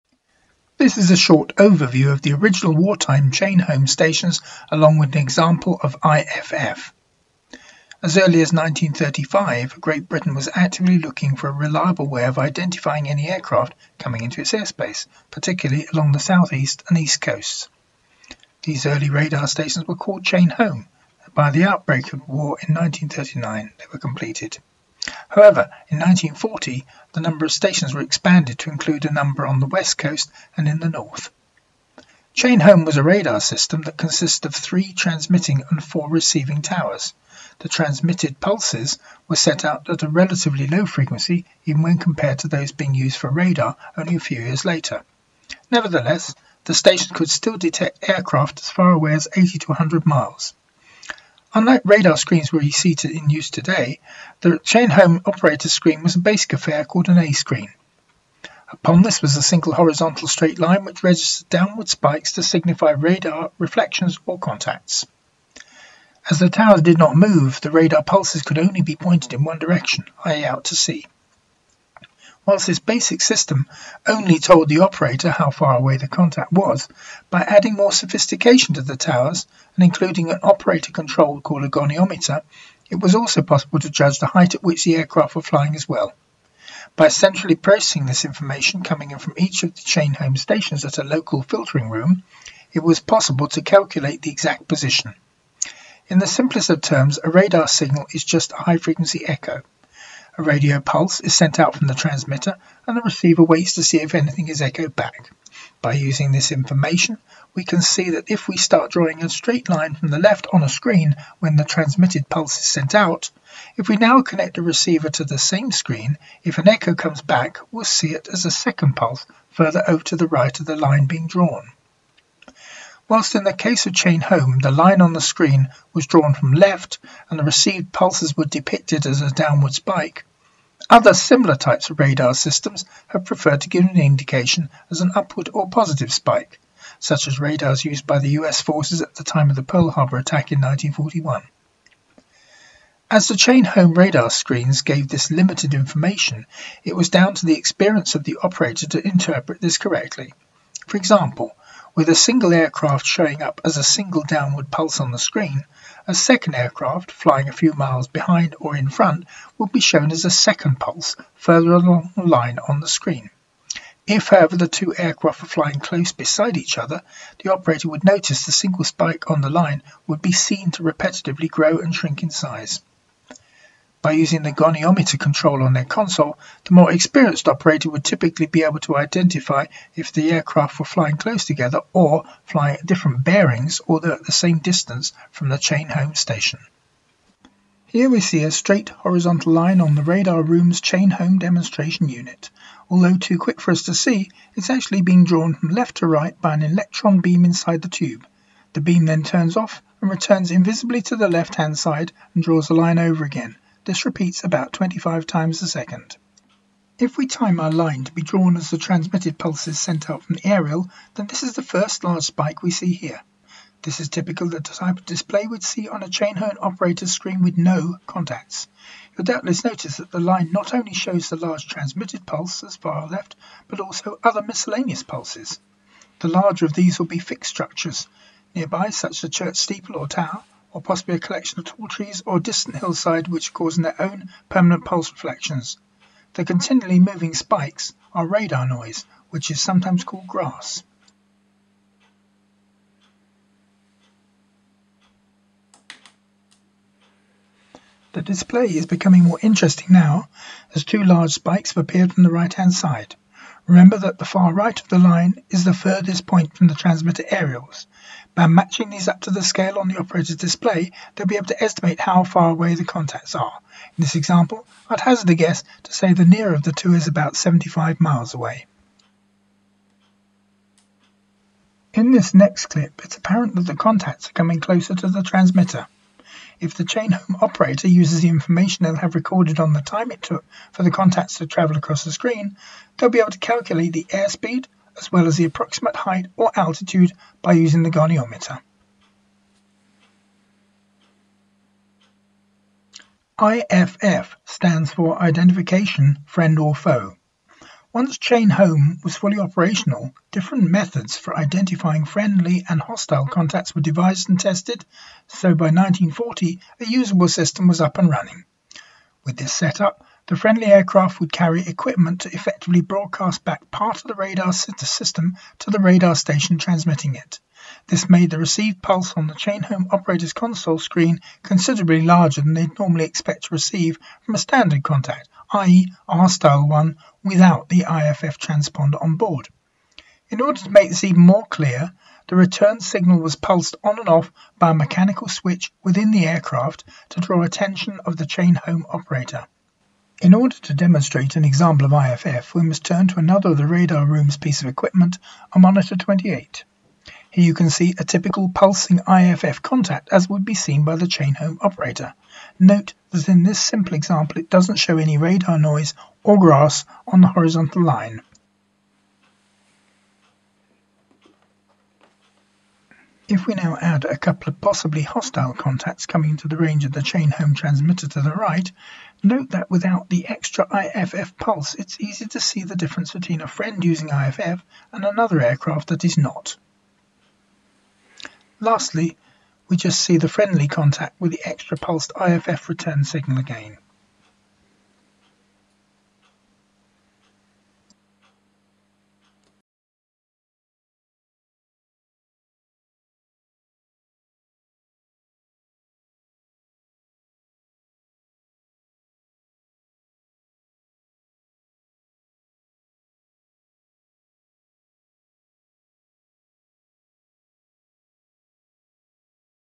1940s Chain Home radar overview
This is a short video giving an overview of how the wartime Chain Home Radar stations operated. We use both diagrams, our working Chain Home demonstrator unit, along with an Air Ministry Monitor 28 indicator unit to demonstrate both the radar and one of the *IFF techniques used at the time.

*Identification - friend or foe.